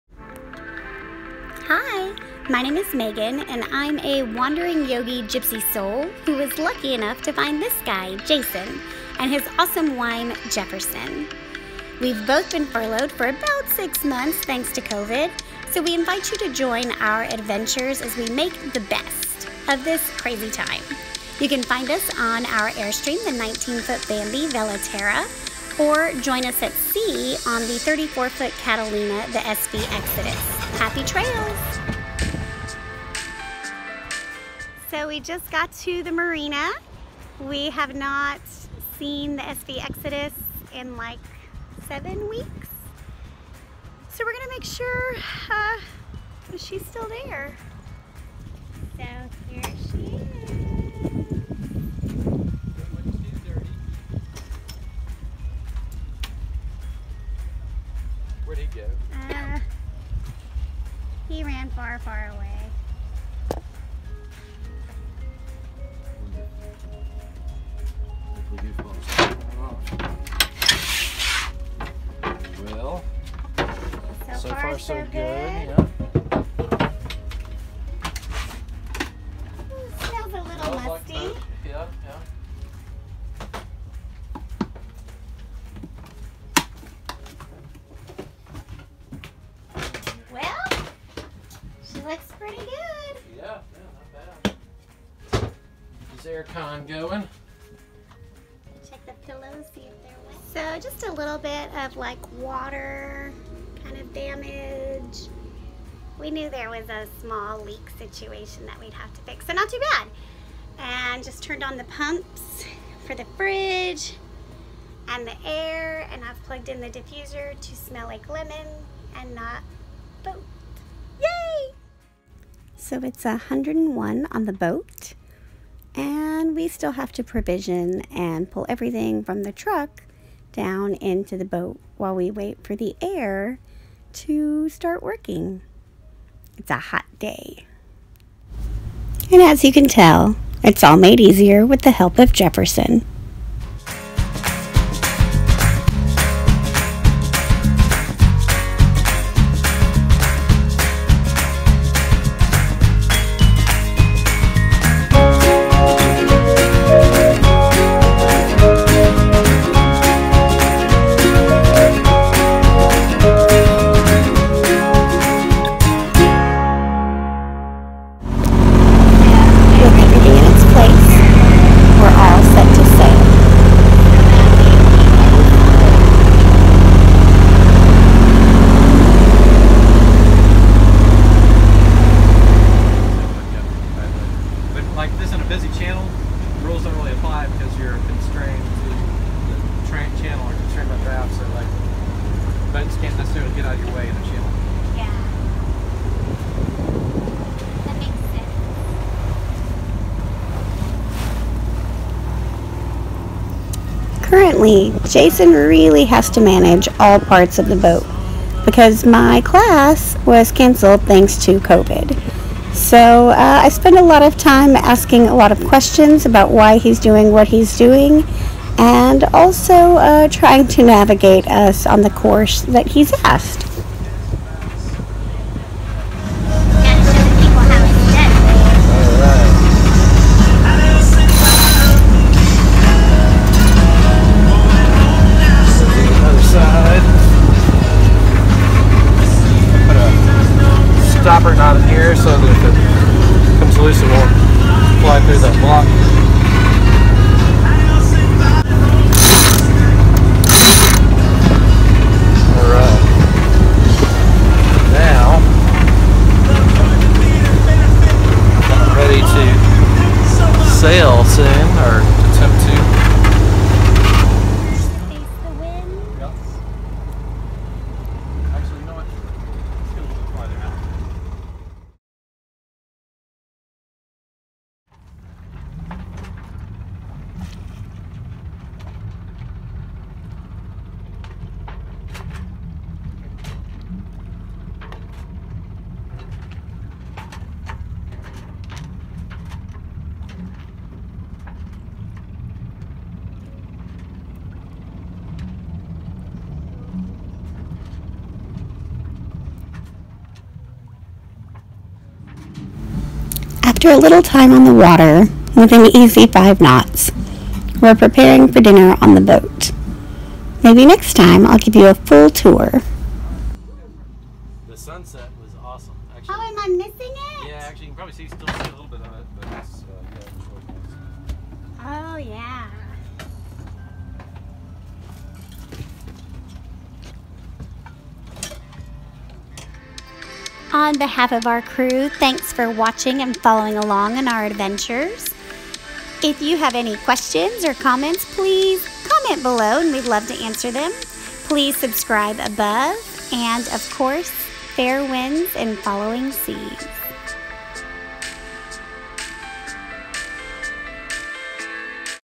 SV Exodus | Sailing | Adventure | Catalina 34
Jason and Megan check in on the SV Exodus after a seven week hiatus. The Catalina 34 might have a leak!